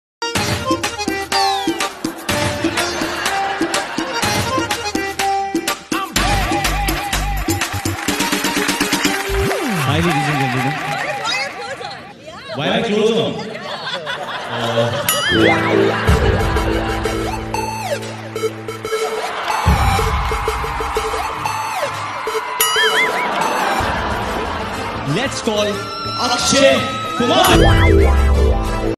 Akshay kumar and Varun Fun in Bath Tub in Lux Award Show|Best Funny Moments Ever
Check out for conneting with me.